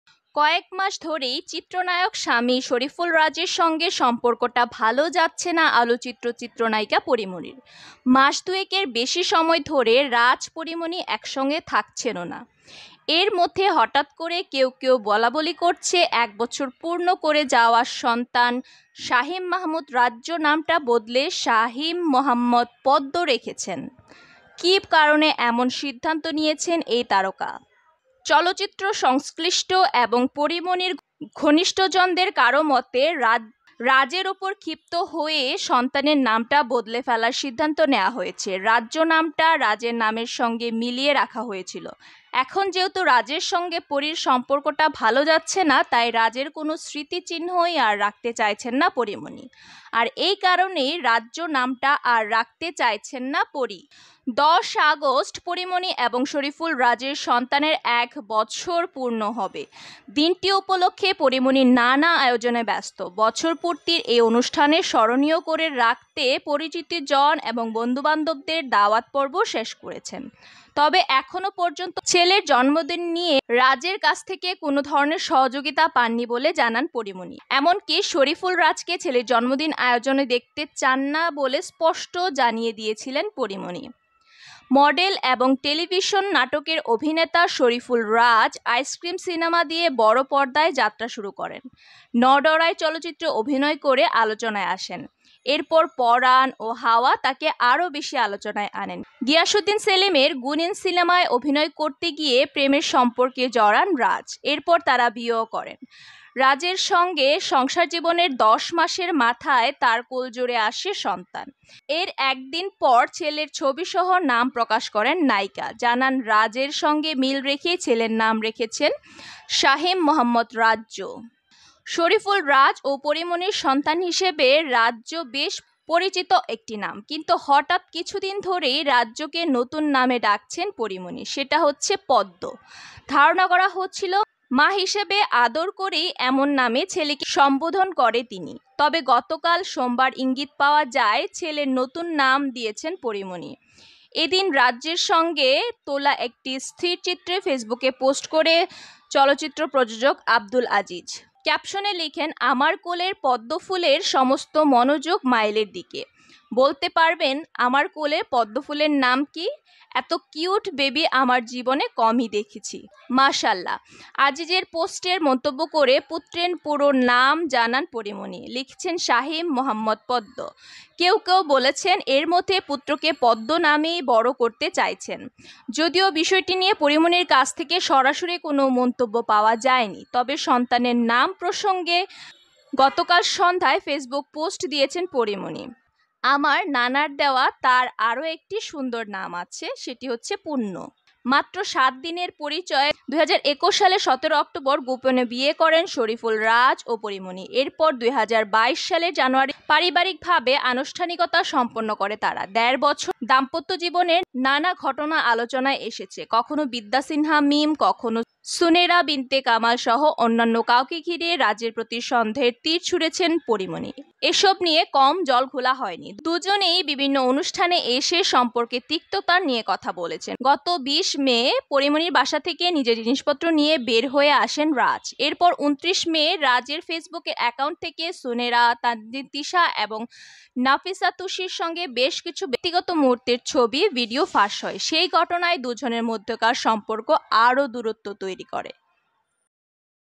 সন্তানের নতুন নাম দিলেন পরীমনি, কারণ কী?
কয়েক মাস ধরেই চিত্রনায়ক স্বামী শরীফুল রাজের সঙ্গে সম্পর্কটা ভালো যাচ্ছে না আলোচিত চিত্রনায়িকা পরীমনির। মাস দুয়েকের বেশি সময় ধরে রাজ–পরীমনি একসঙ্গে থাকছেনও না। এর মধ্যে হঠাৎ করে কেউ কেউ বলাবলি করছেন, এক বছর পূর্ণ করতে যাওয়া সন্তান শাহীম মাহমুদ রাজ্য নামটা বদলে শাহীম মুহাম্মদ পদ্ম রেখেছেন। কী কারণে এমন সিদ্ধান্ত নিয়েছেন এই তারকা?